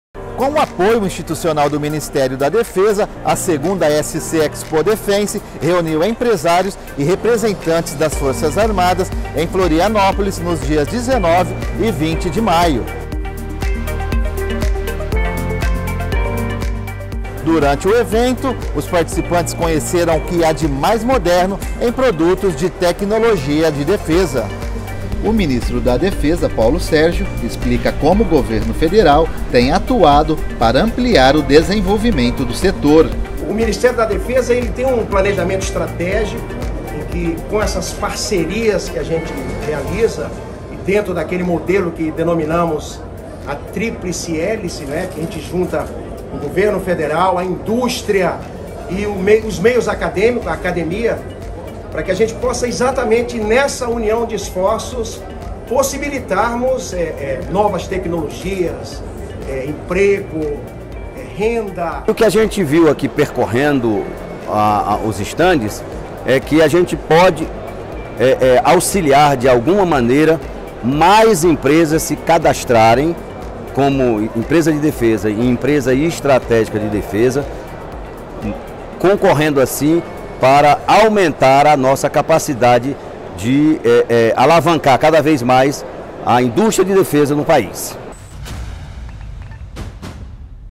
Oportunidades de negócios no setor de Defesa marcam 2ª SC Expo Defense
Durante a 2ª edição da SC Expo Defense - Feira de Tecnologias e de Produtos de Defesa, o Ministro da Defesa, Paulo Sérgio, explicou como o Governo do Brasil tem atuado para ampliar o desenvolvimento do setor. Saiba mais 🎥